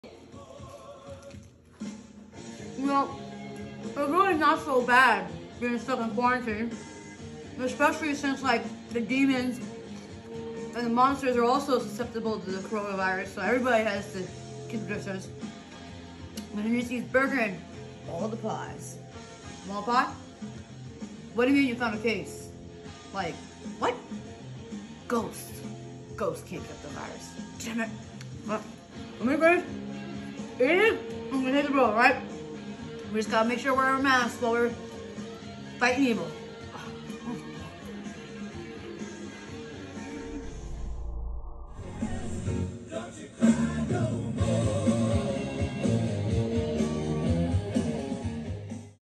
The Brina and Bob Show: hitting the road during the pandemic
This show is a series of episodes done during the Covid 19 Pandemic as a way to bring entertainment during a difficult time in the world, while also providing creative outlet for an actor/director/partner dancer who could no longer dance or interact with others while in sheltering in place during the pandemic. These videos reimagine popular songs, movies, TV shows and holiday icons, if they were made during or for the pandemic.